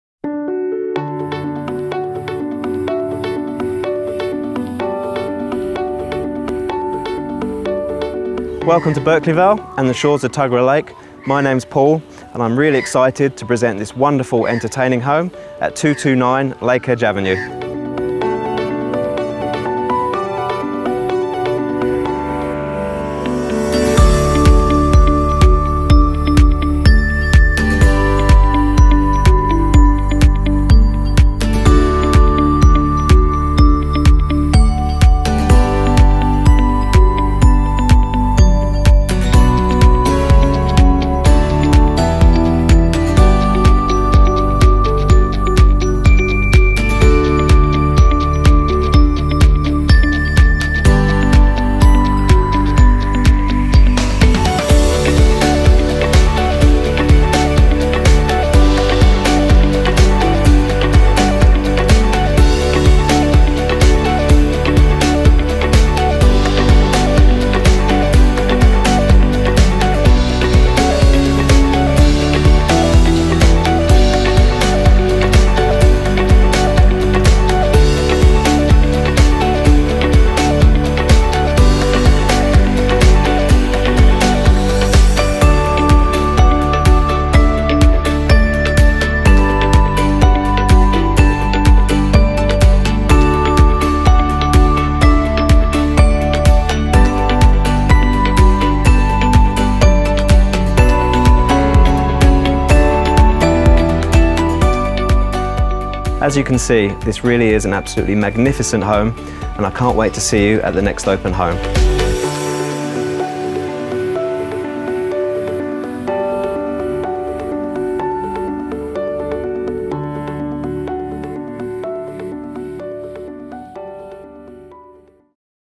229 Lakedge Ave, Berkeley Vale
Amazing single lakefront home for sale auction 4th November 2.30pm.